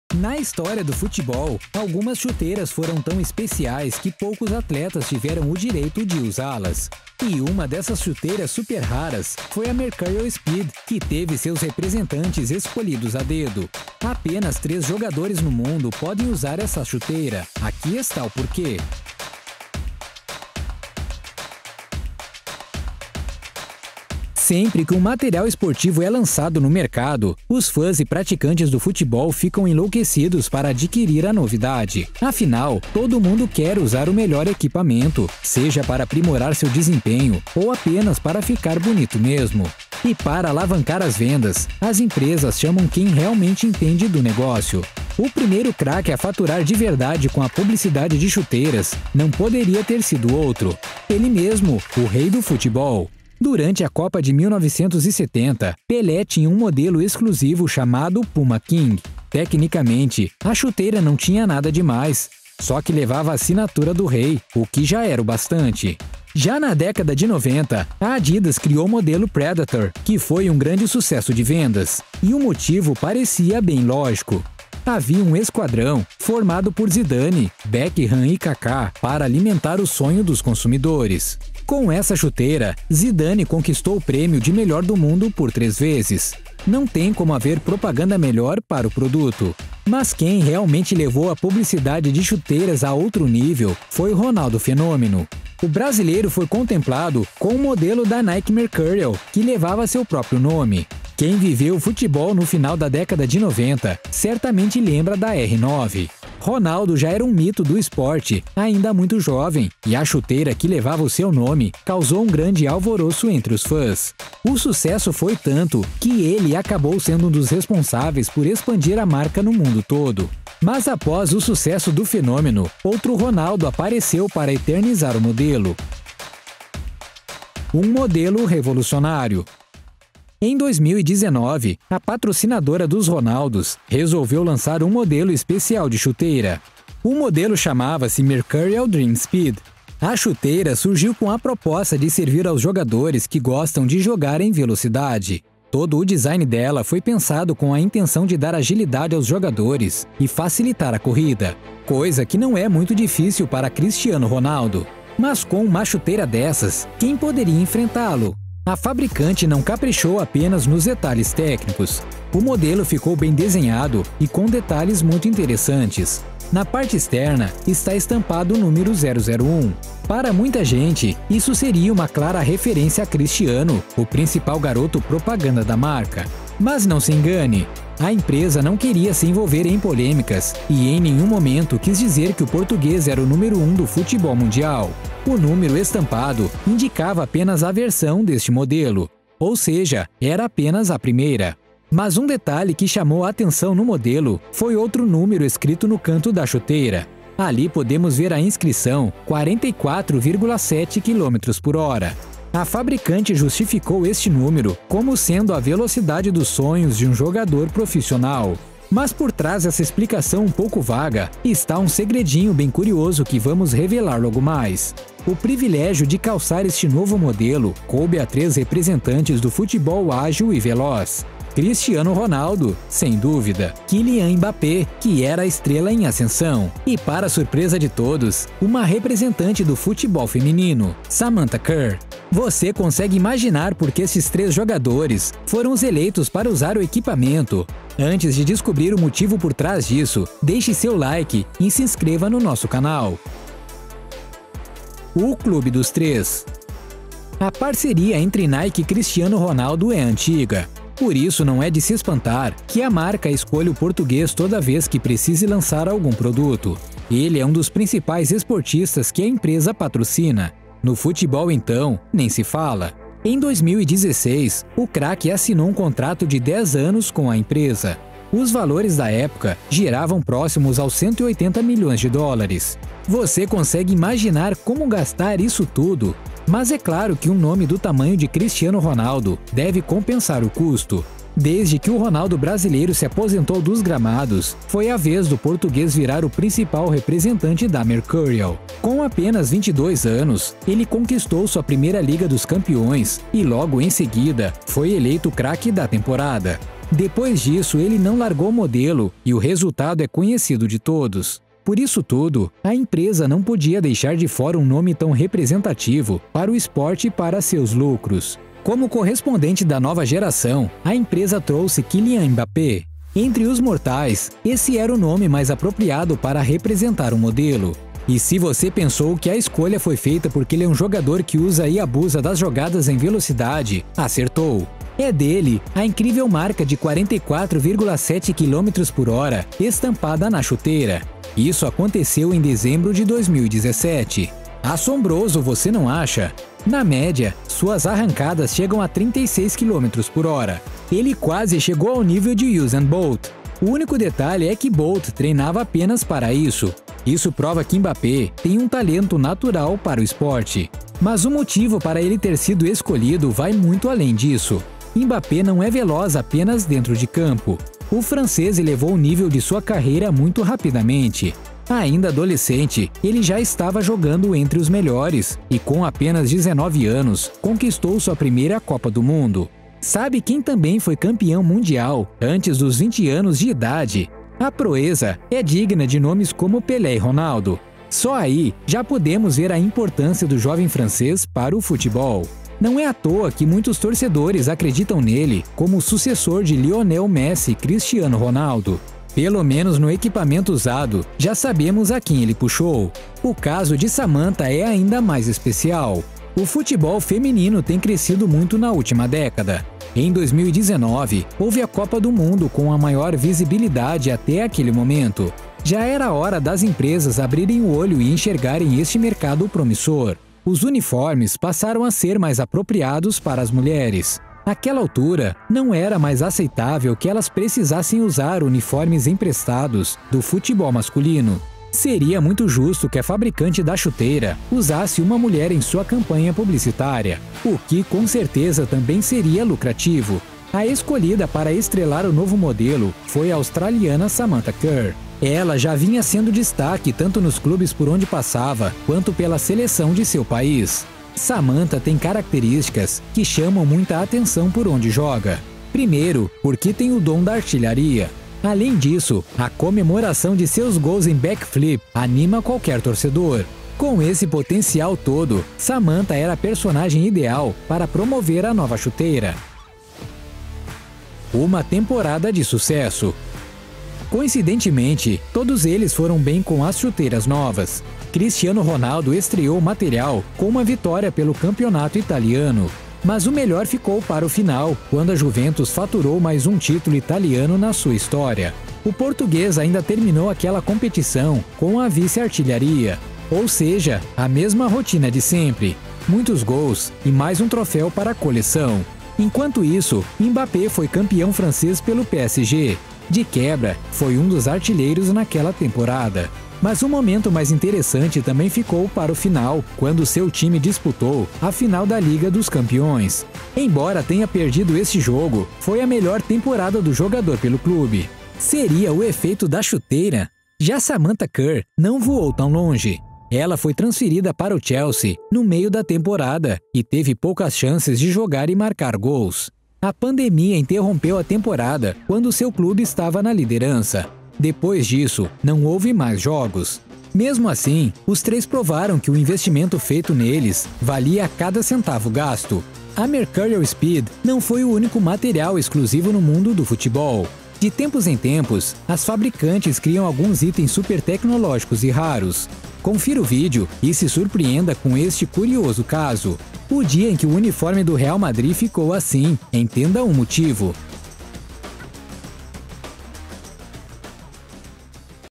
Apenas 3 Jogadores no Mundo podem Usar Essa Chuteira. Aqui Está o Porquê...
Hoje o Torcedor Curioso vai te mostrar a chuteira que apenas 3 jogadores no mundo inteiro podem usar!

Alguns jogadores já foram contemplados com chuteiras que foram produzidas com uma característica ou um design próprio, as empresas fazem isso para alavancar as vendas, e um dos primeiros a ser beneficiado com um modelo desses foi o Rei Pelé… Mas o que vamos te mostrar aqui hoje, é que existe um modelo tão especial, que apenas 3 jogadores no mundo podem jogar com ela… Veja no vídeo!